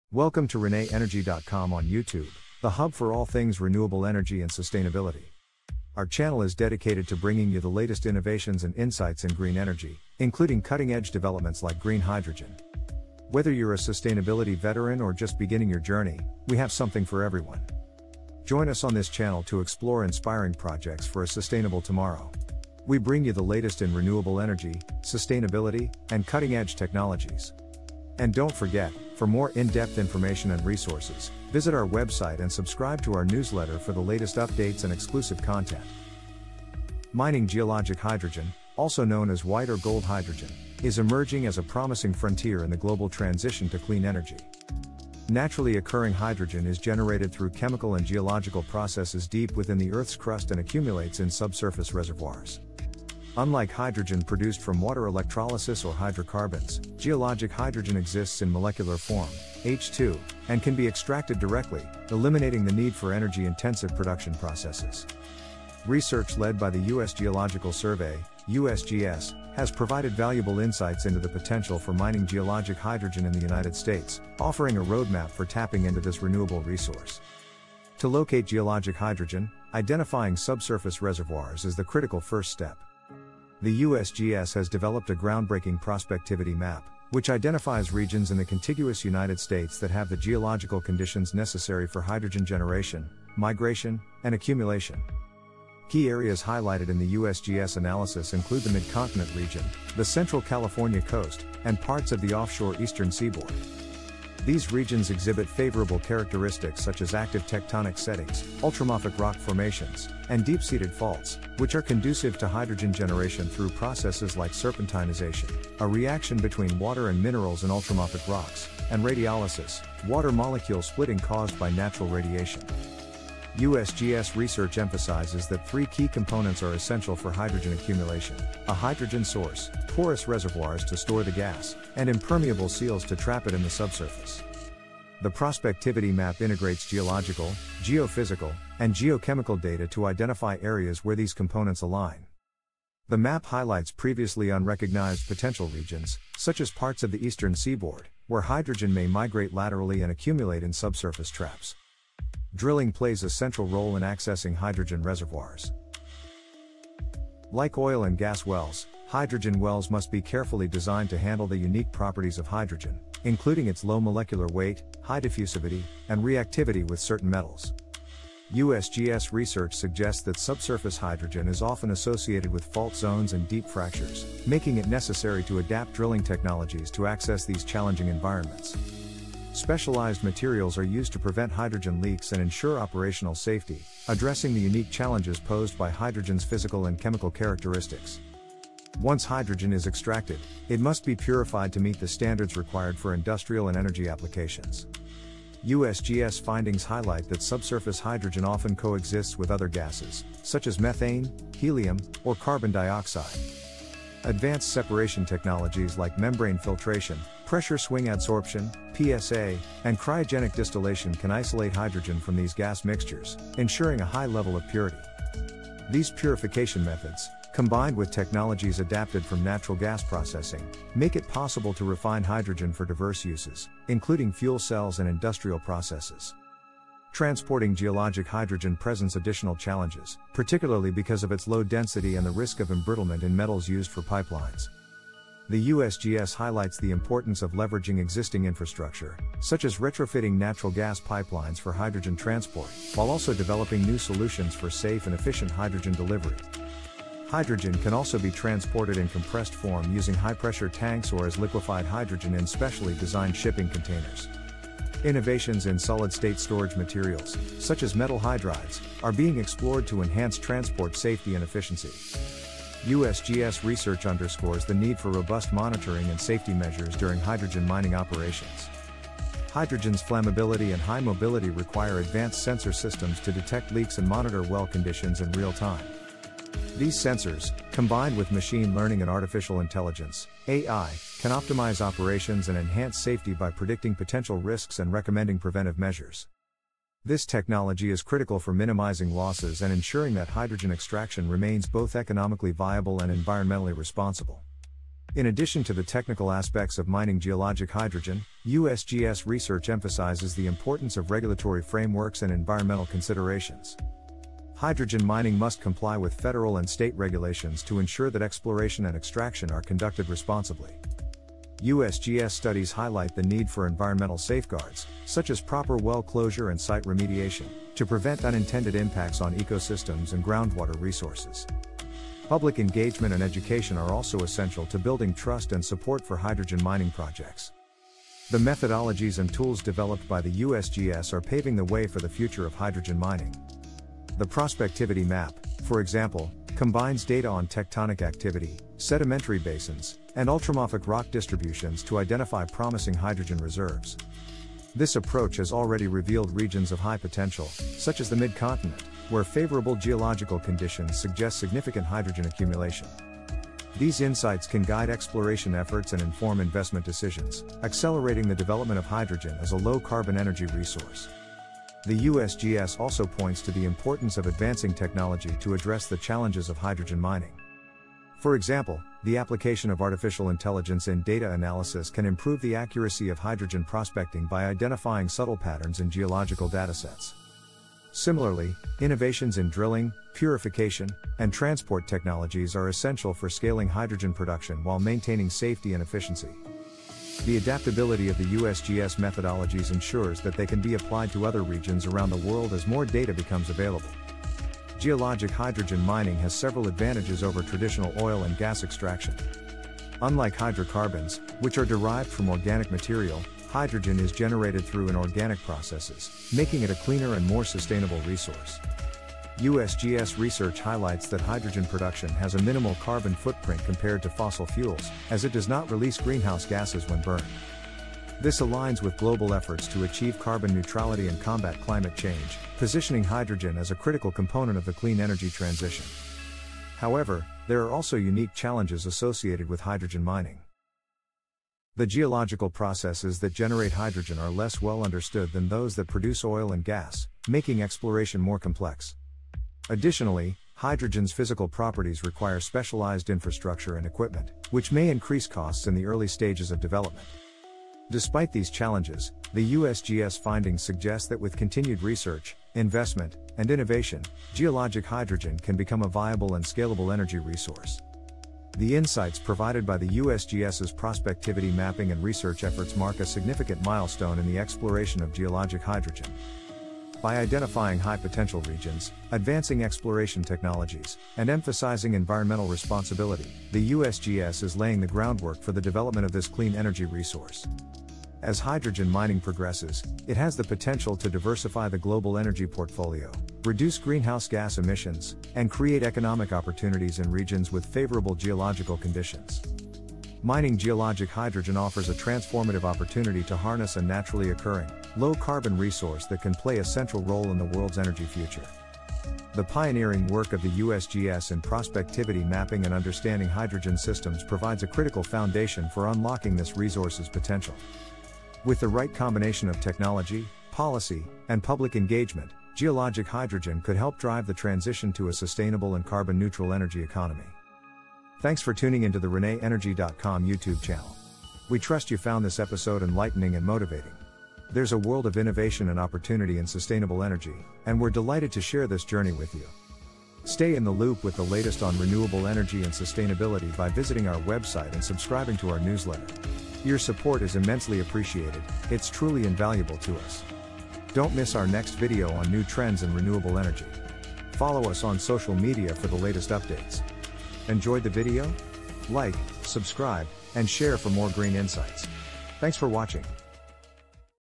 Geologic Hydrogen Mining - The Future of Clean Energy
Explore how geologic hydrogen, or naturally occurring hydrogen, is being mined as a sustainable energy resource. With insights from groundbreaking research by the U.S. Geological Survey (USGS), this video dives into the science, technology, and future of hydrogen extraction. Discover how advanced exploration techniques, AI-driven systems, and innovative transport solutions are unlocking this untapped, low-carbon energy source.

🔬 What You’ll Learn in This Video:
✅ What geologic hydrogen is and how it forms in the Earth's crust
✅ USGS prospectivity mapping and high-potential regions in the U.S.
✅ Technologies for hydrogen extraction, purification, and transport
✅ The role of AI, IoT, and advanced sensors in hydrogen mining
✅ Environmental and regulatory considerations for responsible development
✅ Future innovations transforming hydrogen mining

💡 Why This Matters:
Geologic hydrogen offers a clean, naturally occurring energy resource that could transform the global energy landscape. By reducing greenhouse gas emissions and diversifying energy sources, hydrogen mining aligns with global sustainability goals and supports the transition to a carbon-neutral future.

🔔 Don’t miss out on the latest clean energy insights! 🚀